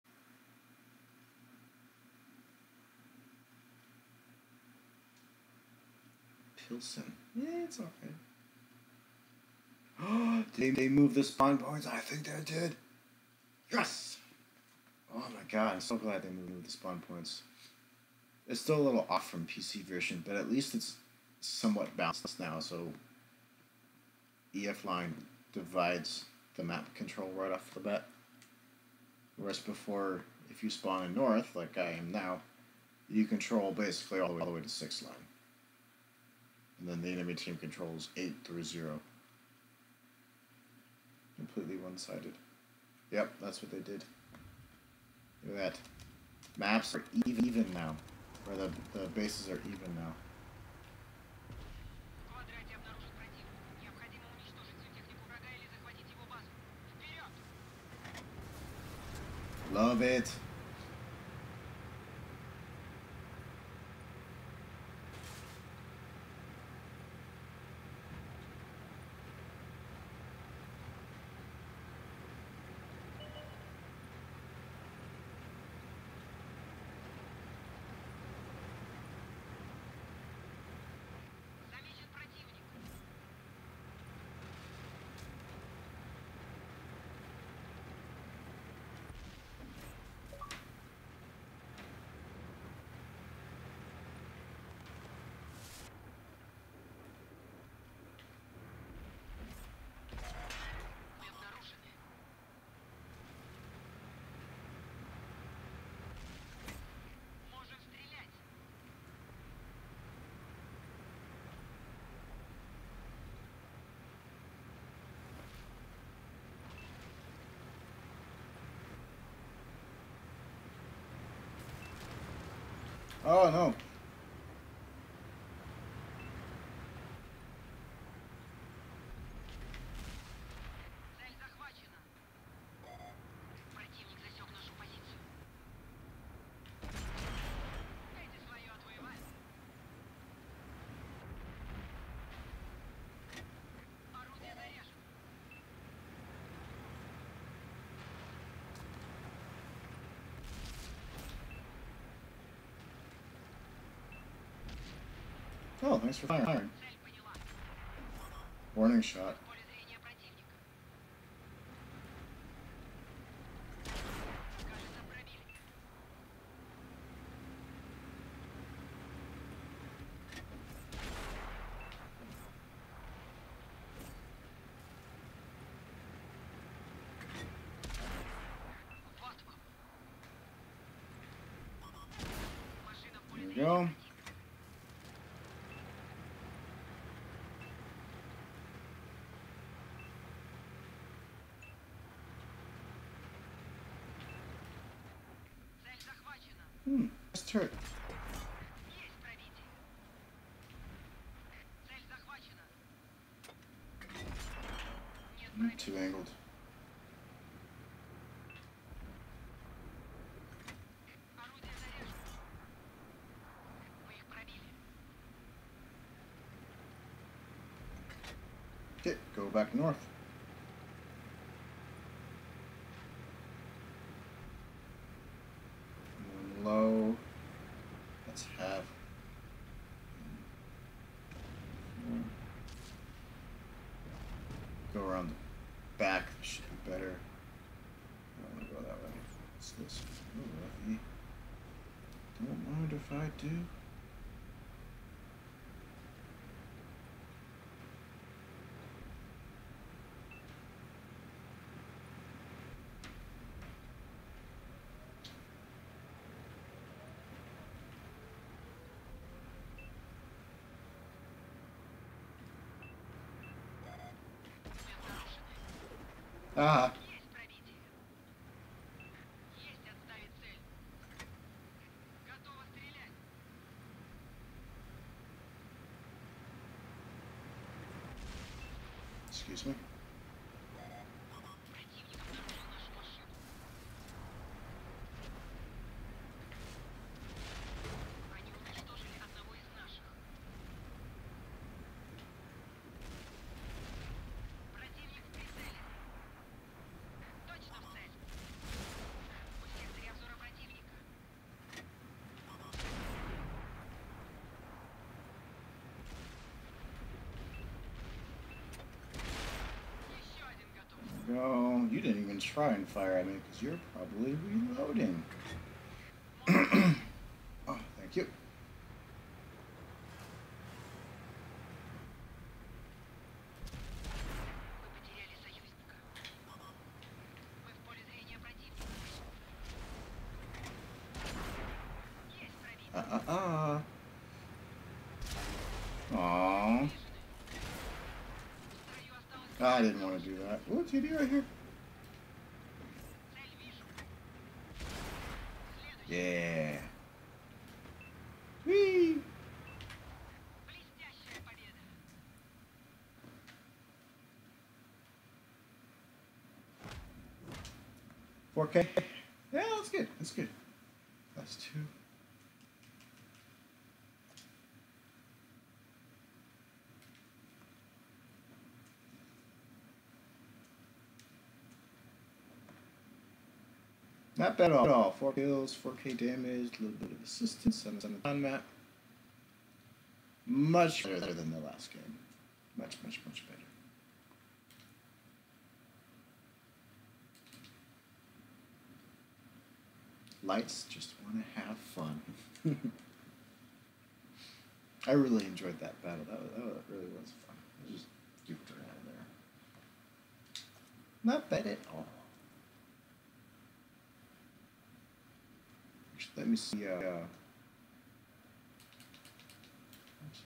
Lights Just Want To Have Fun!
5.3k Damage (Combined) on Pilsen (SPAWN POINTS FINALLY FIXED!!!) Mainly showing this one because I just had a lot of fun playing it.